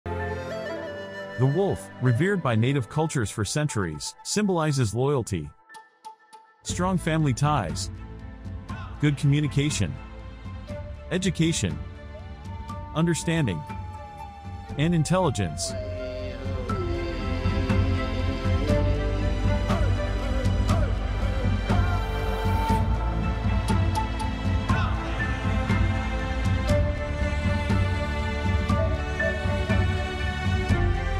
Tribal Music | Wolf Spirit 🐺 Strengthening Family Bonds with Native Wisdom | Spiritual Meditation
Immerse yourself in the timeless embrace of Native American wisdom and the captivating allure of the Wolf spirit with our latest video. 🐺🌾 Let the soothing sounds of tribal music transport you to a world where nature's rhythms intertwine with ancient traditions.

🎵 Mesmerizing Tribal Music: Immerse yourself in the soulful sounds of traditional Native American instruments like drums, flutes, and rattles. Each note resonates with the heartbeat of the earth.

(Note: This video is a creative visualization experience inspired by Native American symbolism and values. We honor and respect the cultures and traditions being depicted.)